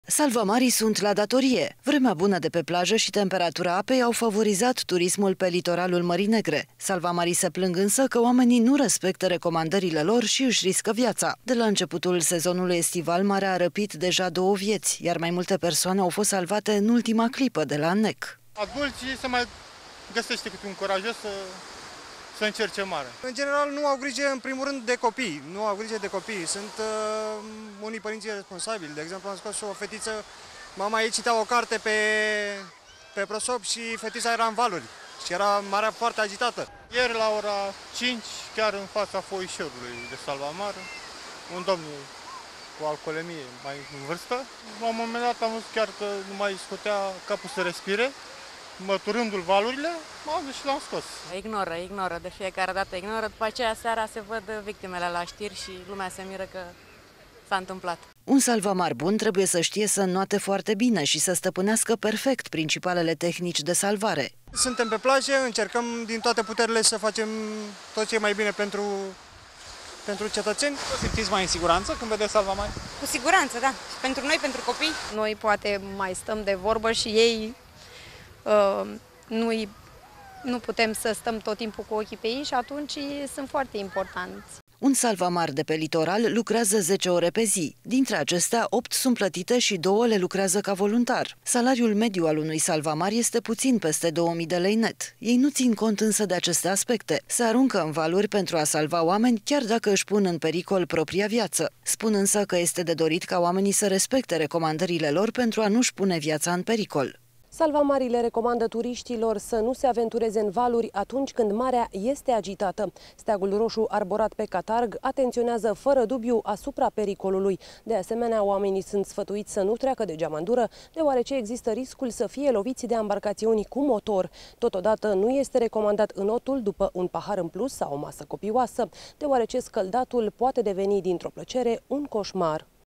Lupta salvamarilor cu turiștii imprudenți
Salvamarii sunt la datorie! Vremea bună de plajă și temperatura apei au favorizat turismul pe litoralul Mării Negre. Salvamarii se plâng, însă, că oamenii nu respectă recomandările lor și riscă viața. De la începutul sezonului estival, marea a răpit deja două vieți, iar mai multe persoane au fost salvate în ultima clipă de la înec.

VOX "Adulții se mai găsește câte un curajos să încerce marea."

VOX salvamar: "În general nu au grijă de copii. Sunt unii părinți iresposabili, de exemplu am scos o fetiță, mama ei citea o carte pe prosop și fetița era-n valuri și era marea foarte agitată."

VOX "Ieri, la ora cinci, chiar în fața foișorului de salvamar, un domn cu alcoolemie, mai în vârstă, la un moment am văzut că nu mai scotea capul să respire, măturându-l valurile. L-am scos."

VOX "Ignoră și după aceea, seara, se văd victimele la știri și lumea se miră că s-a întâmplat."

Un salvamar bun trebuie să știe să înoate foarte bine și să stăpânească perfect principalele tehnici de salvare.

VOX "Suntem pe plajă, încercăm din toate puterile să facem tot ce e mai bine pentru cetățeni."

VOX "- Vă simțiți mai în siguranță când vedeți salvamarii? - Cu siguranță, da. Pentru noi, pentru copii."

Un salvamar de pe litoral lucrează zece ore pe zi. Dintre acestea, opt sunt plătite și două le lucrează ca voluntar. Salariul mediu al unui salvamar este puțin peste două mii de lei net. Ei nu țin cont, însă, de aceste aspecte. Se aruncă în valuri pentru a salva oameni, chiar dacă își pun în pericol propia viață. Spun, însă, că este de dorit ca oamenii să respecte recomandările lor, pentru a n-și pune viața în pericol.

ALEXANDRA GEORGESCU, reporter "Salvamarii le recomandă turiștilor să nu se aventureze în valuri, atunci când mare este agitată. Steagul roșu arborat pe catarg atenționează fără dubiu asupra pericolului. De asemenea, oamenii sunt sfătuiți să nu treacă de geamandură, deoarece se expun pericolului de a fi loviți de ambarcațiunile cu motor. Totodată, nu este recomandat înotul după un pahar în plus sau o masă copioasă, deoarece plăcerea scăldatului se poate transforma într-un coșmar."